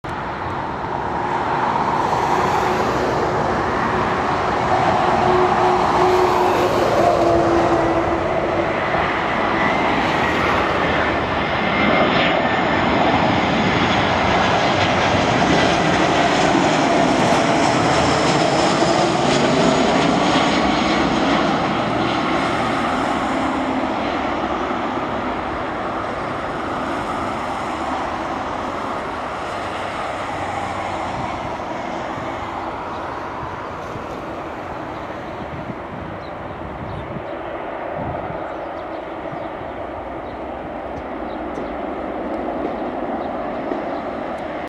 Throwback! Germanwings Airbus A319-112 D-AKNN Landing at Berlin Airport
Dieser fliegt jetzt beim Nachfolger @eurowings

Airline: Germanwings
IATA Code: 4U
ICAO Code: GWI
Callsign: GERMANWINGS
Aircraft type: Airbus A319-112
Registration: D-AKNN

More Channels

and-